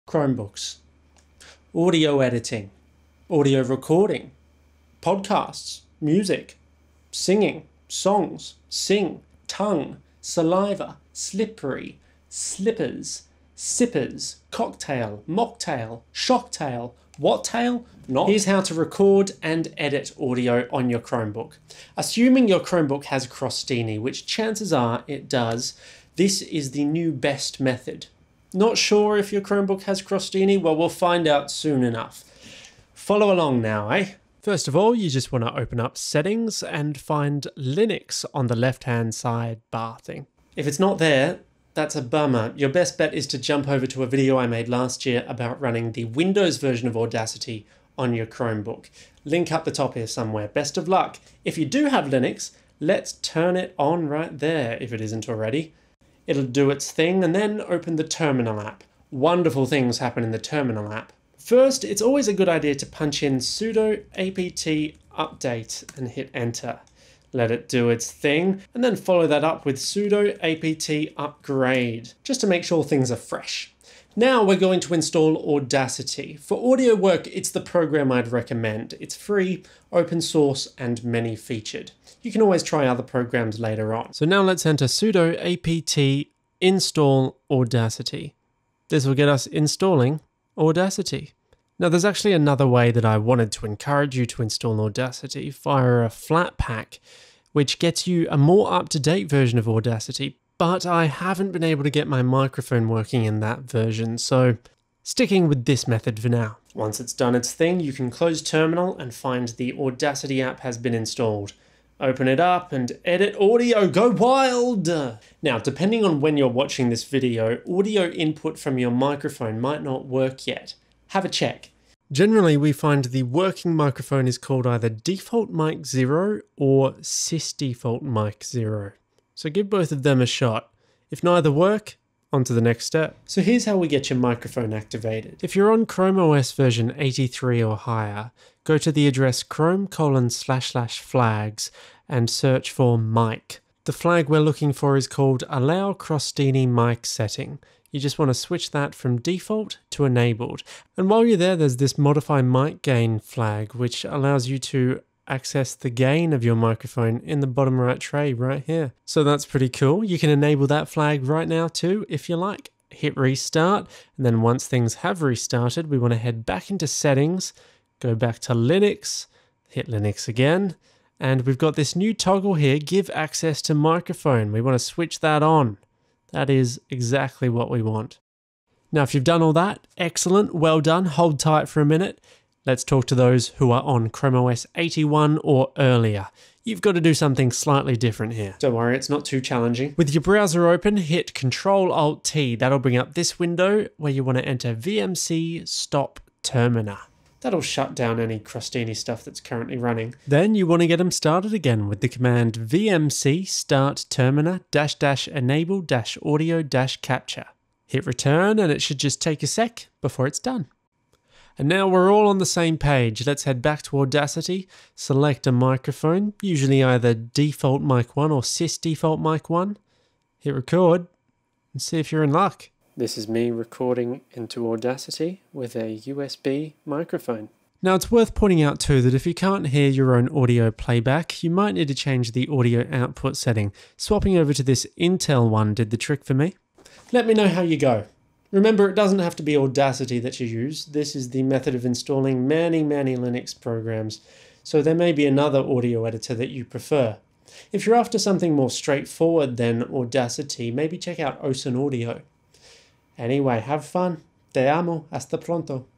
How to RECORD & EDIT AUDIO on a Chromebook (2020)
I've waited a looong time to make this video. I was waiting for Crostini audio capture to be more accessible. And it finally is, so here we go!